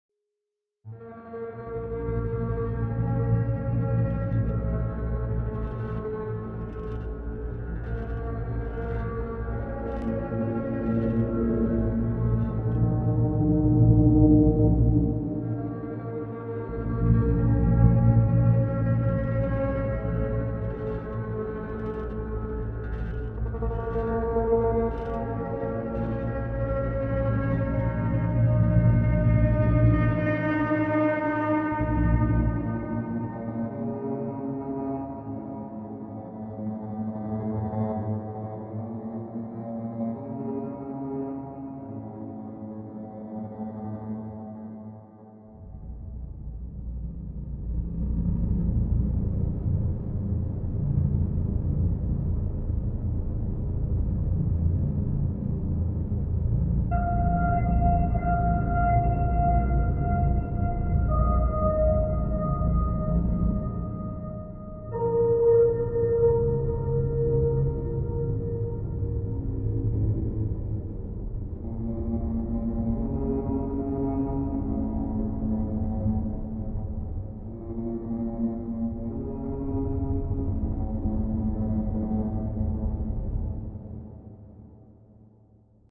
Epic Music for Dark Chaos
A cinematic, Atmospheric, Dark, Sad, epic, beautiful arrangement. Got inspired by Hans Zimmer soundtracks. More info on the description.

- Please do not claim the music to be yours, We are trying to help Creators.